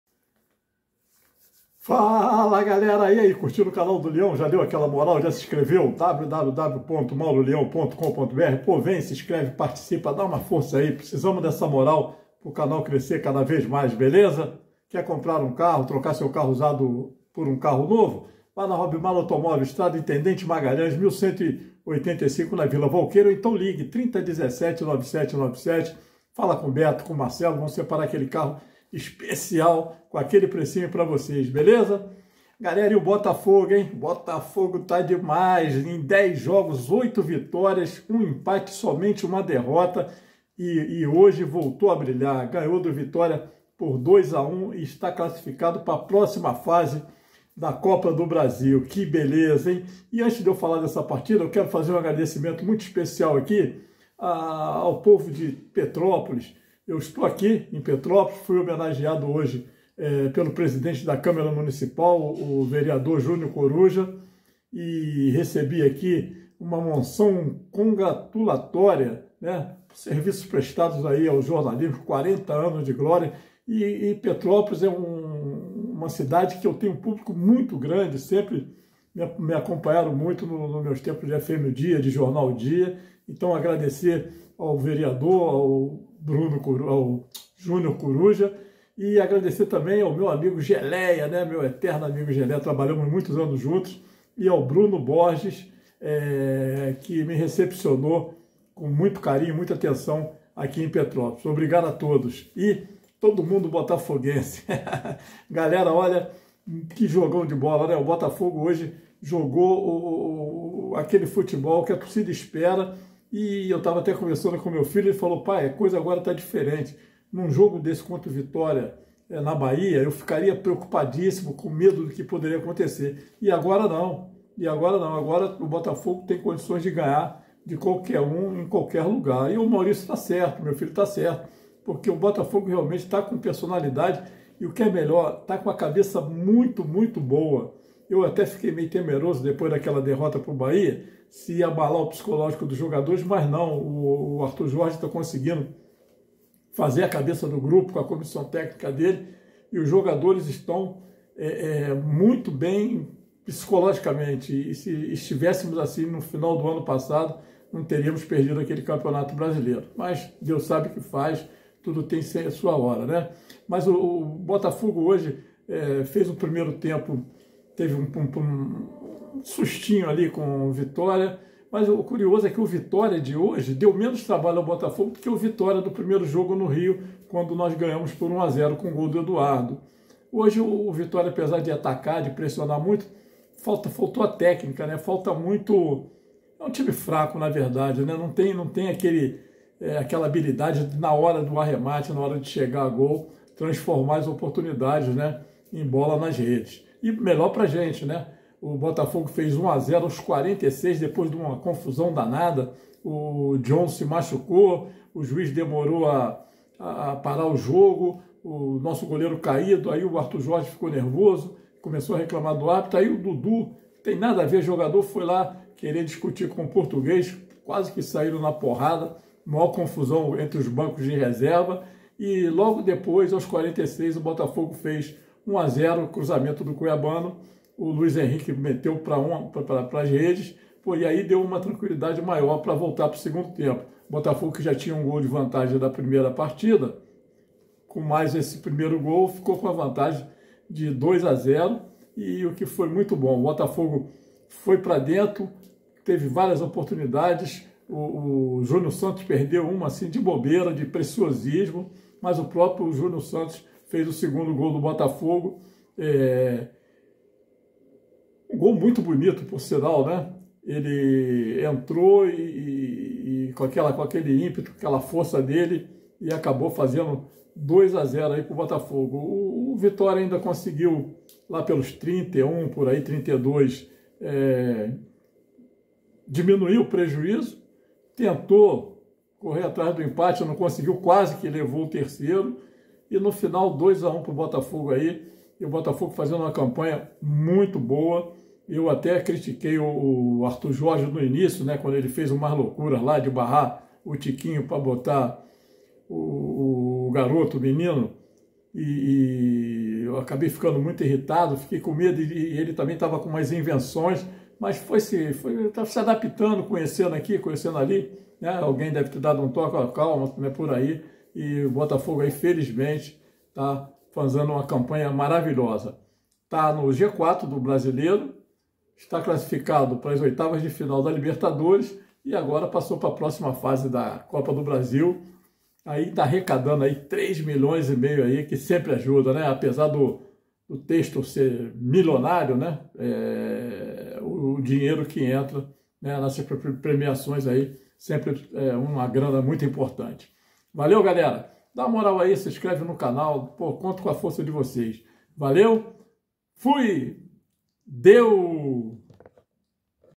FOGÃO SE CLASSIFICA NA COPA DO BRASIL: METEU 2 A 1 NO VITÓRIA, EM SALVADOR!!!
SÃO OITO VITÓRIAS E UM EMPATE NOS ÚLTIMOS 10 JOGOS. LUIS HENRIQUE E JÚNIOR SANTOS MARCARAM.PARA O BOTAFOGO.